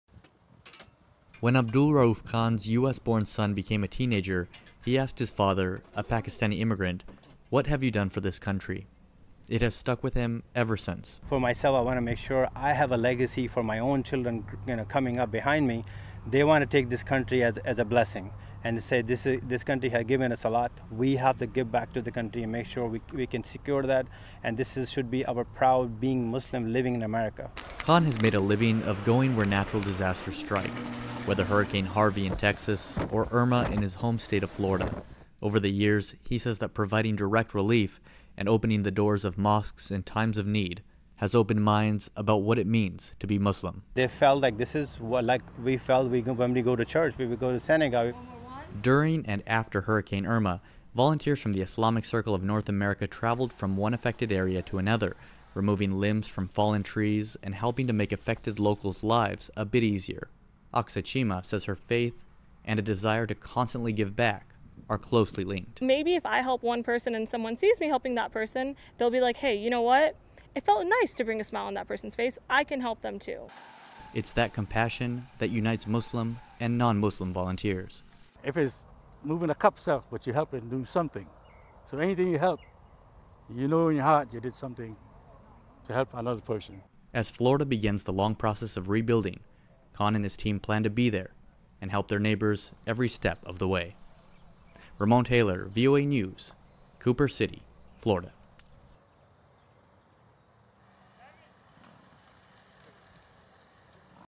For Muslim relief workers, faith & charity form inextricable bond
By Ramon Taylor, September 15, 2017

COOPER CITY, FLORIDA — When Hurricane Irma battered the Florida Coast, volunteers of the Islamic Circle of North America (ICNA) arranged shelter. After it passed, they provided relief — from flood-damaged homes in Naples to uprooted tree trunk clearings in Cooper City, Florida.